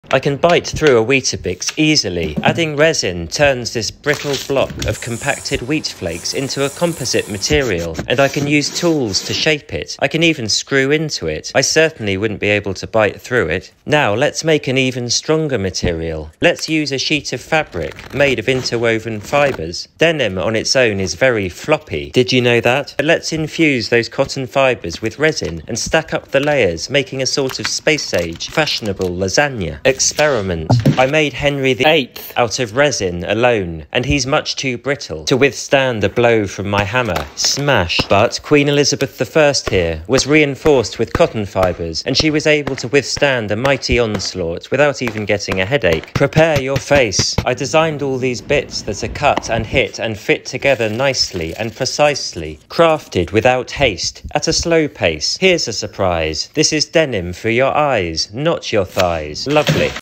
Turning Weetabix into a composite material
I turned Weetabix into a composite material by infusing it with resin. I enjoyed doing this. I also biting through the normal Weetabix using my mechanical smiling machine.